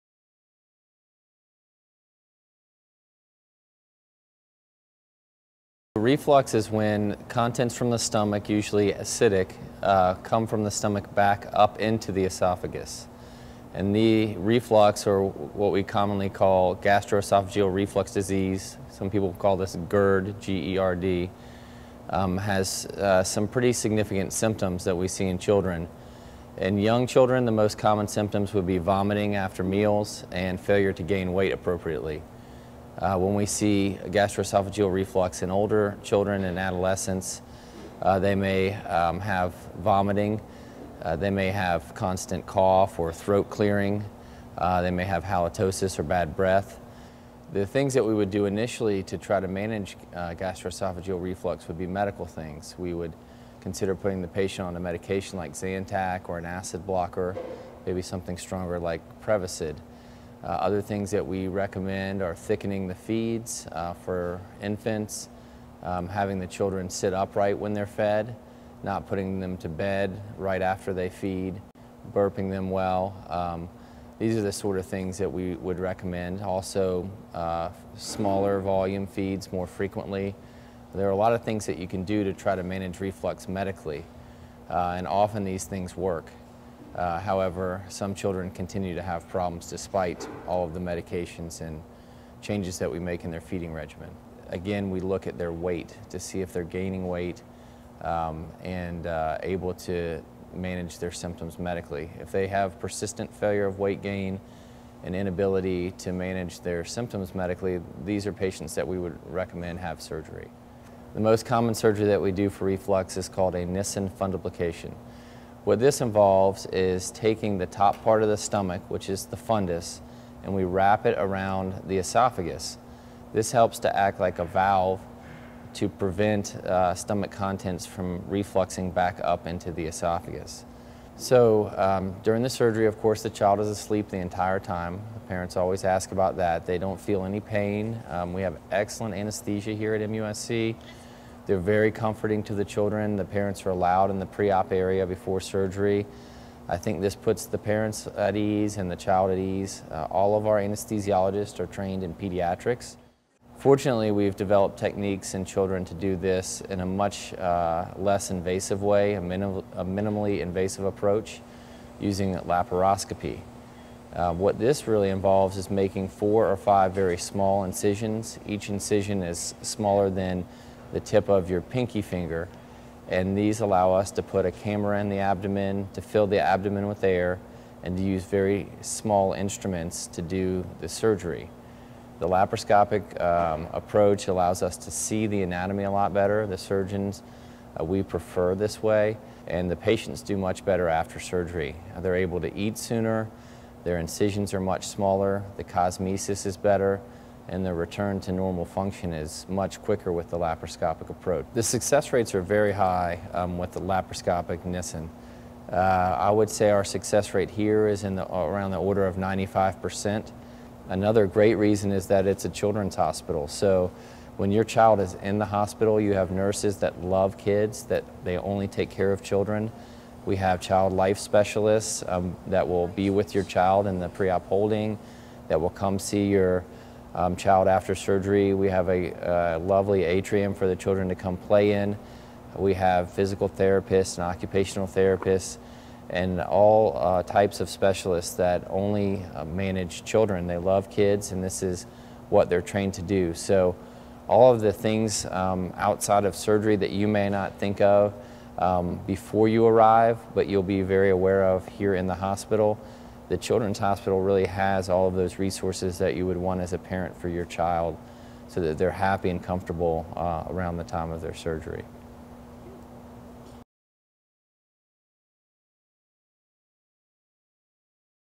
Reflux Surgery In Children - Frequently Asked Questions
Dr. Chris Streck, Pediatric Surgeon at the MUSC Children's Hospital answers the most frequently asked questions about reflux and surgery in children. MUSC offers expertise in the minimally invasive surgery for reflux, also known as a nissen fundoplication. Find out symptoms of reflux, what's involved in the surgery, and what to expect with hospital stay and recovery.